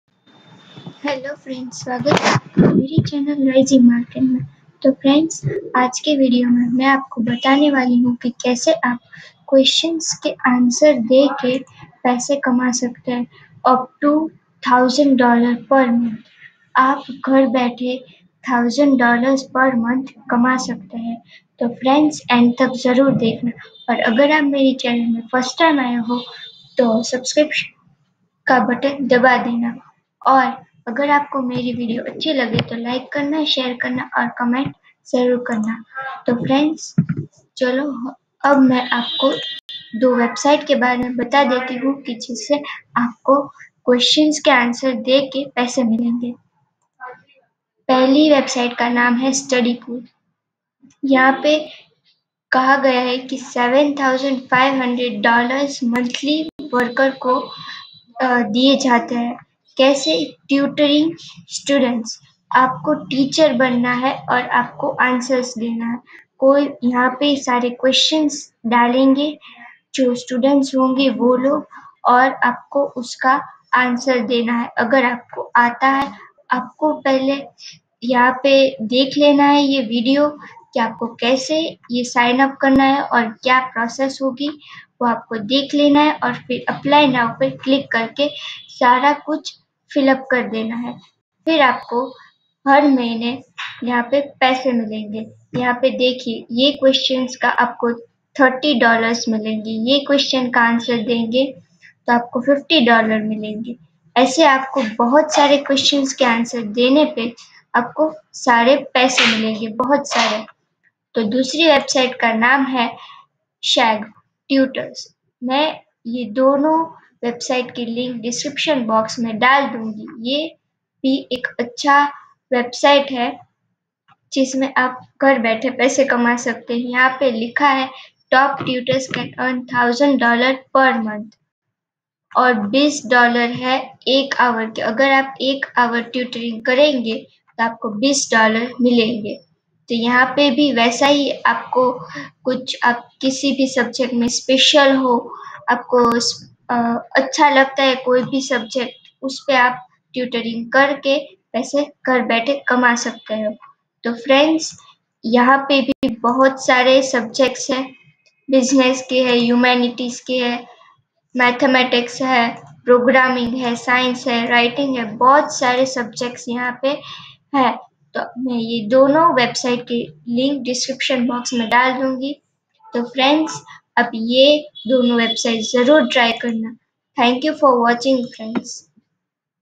EARN MONEY BY ANSWERING THE QUESTION | EARN MONEY FASTER🔥🔥
Hello friends,
   In this video u will see how to earn money by just answering the question.
there 4 website which will help u to earn money online ...

studypool tutor
chegg tutors
answer the question website
earn by answering questions
earn by answering survey
earn by answering questions online